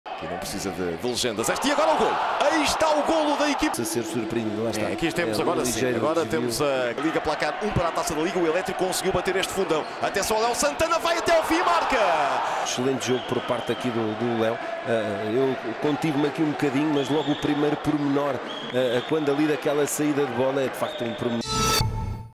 Liga Placard (8.ª Jornada): Eléctrico FC 2-0 AD Fundão
Liga Placard
8.ª Jornada
Eléctrico FC 2-0 AD Fundão